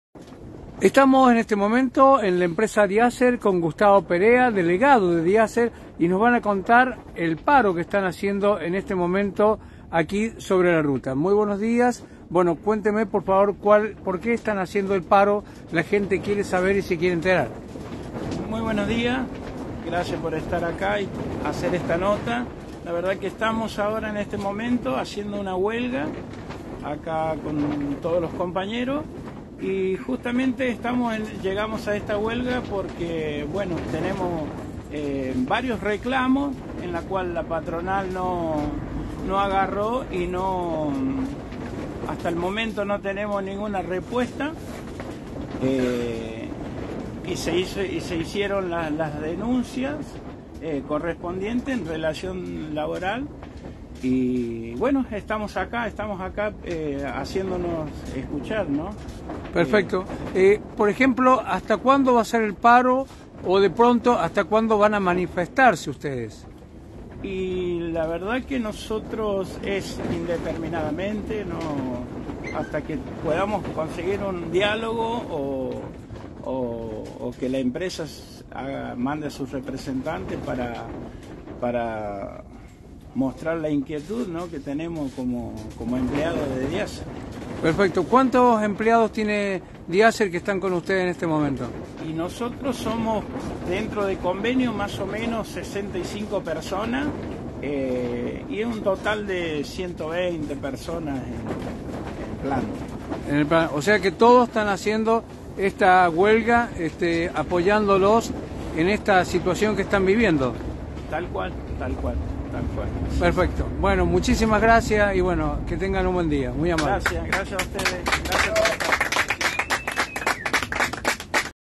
⭕Reclamo de encuadre y derecho laboral en Empresa Diaser
❌Operarios reclaman mejoras y condiciones laborales.